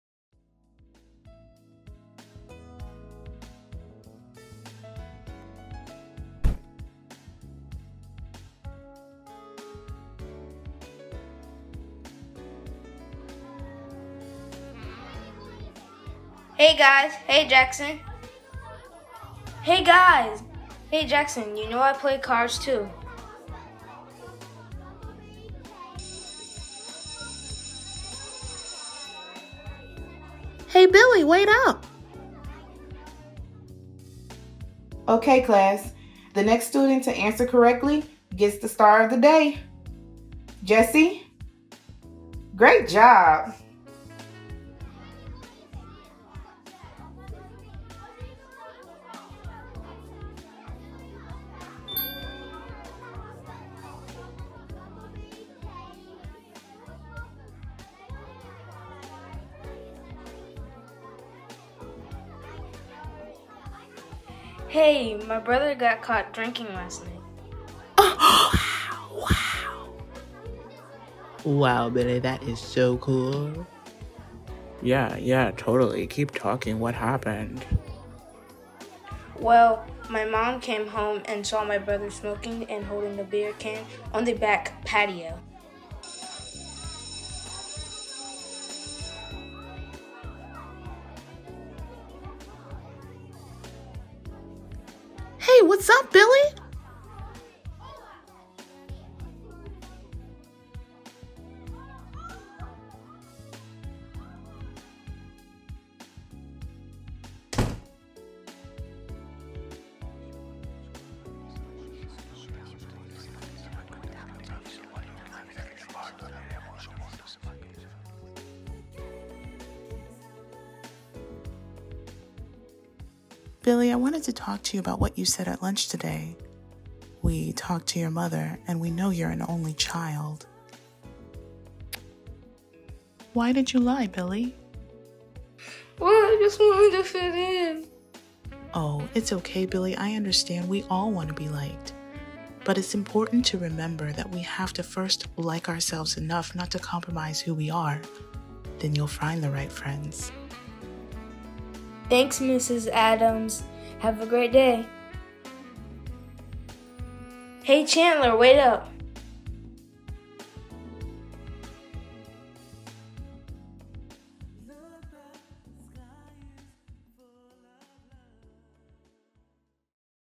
ANIMATIC-Why Lie Billy
This is a student final project for Visual Storytelling and Development. The story tells of what many children experience everyday in their school environment. We should always encourage our children to be themselves.